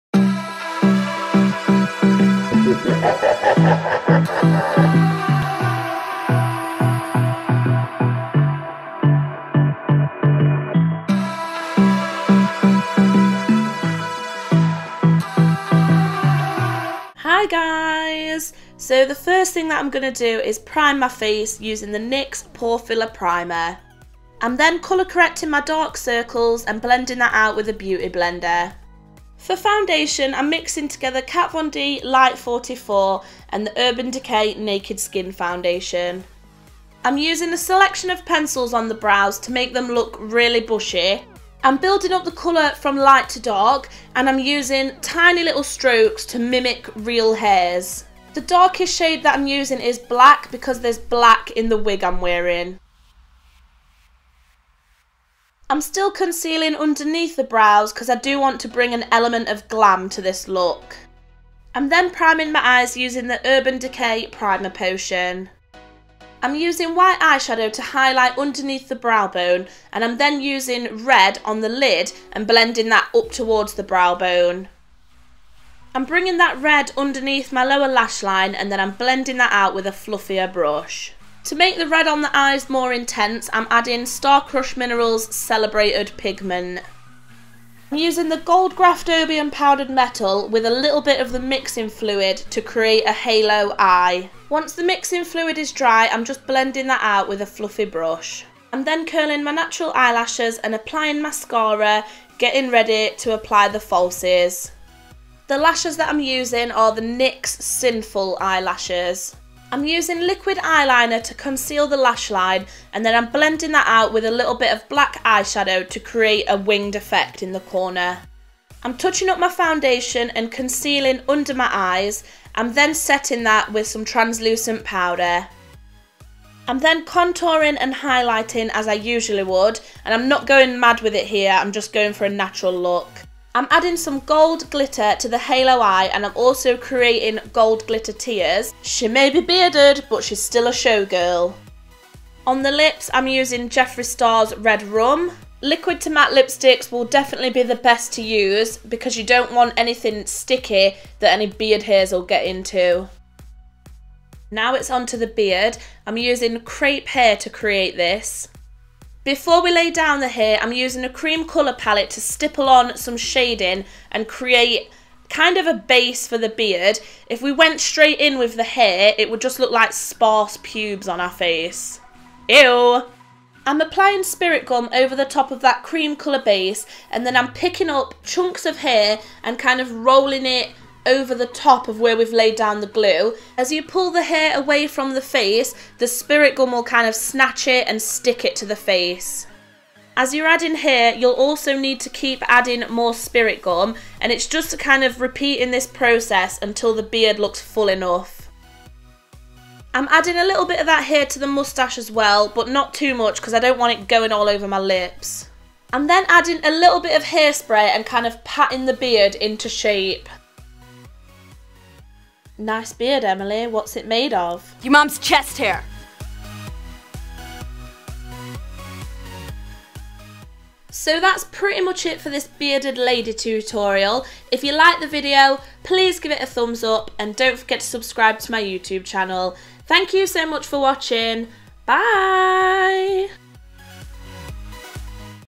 Bearded Lady Halloween Makeup Tutorial
Hey guys! Todays looks is this glam bearded lady! Ideal for any of you queens out there wanted to learn how to lay down fake hair! Hope you're enjoying the 31 days so far!!

Mehron spirit gum
Mehron crepe hair
Mehron cream blend/mask cover palette
NYX taupe eyebrow pencil
NYX black micro brow pencil
NYX brown perfection slide on glide on pencil
NYX sinful lashes
Kat Von D light 44 foundation
Kat Von D tattoo liner in trooper
Kat Von D thunderstruck eyeshadow
Sarah Hill holy grail glitter
Graftobian gold powdered metal and magic set fluid
Star Crushed Minerals celebrated pigment
Jeffree Star Cosmetics red rum lipstick
Sugarpill tako and love+
Morphe Brushes picasso palette

VanityVenom